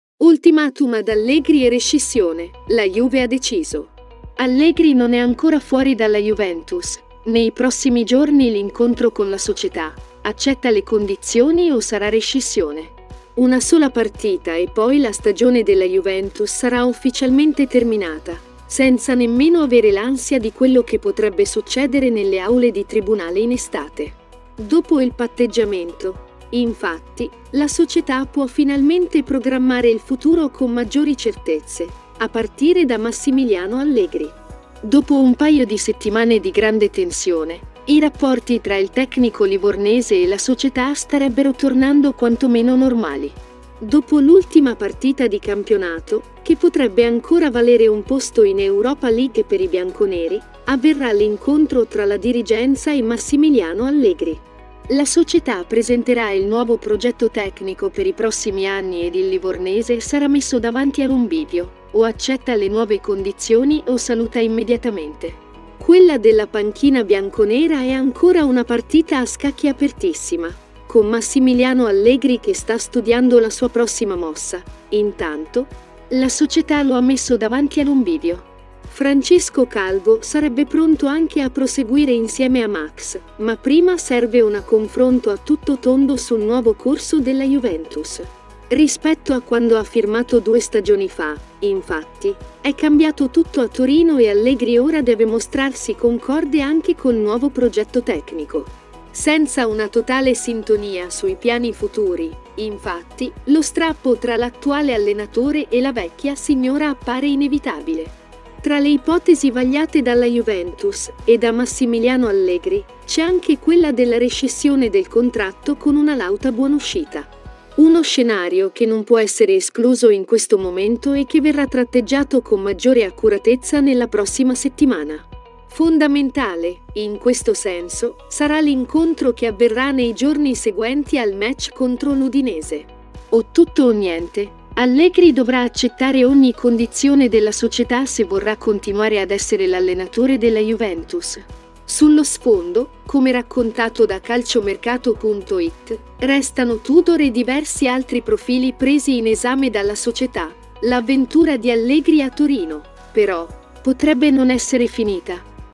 Allegri e rescissione: la Juve ha deciso
Allegri non è ancora fuori dalla Juventus, nei prossimi giorni l’incontro con la società: accetta le condizioni o sarà rescissione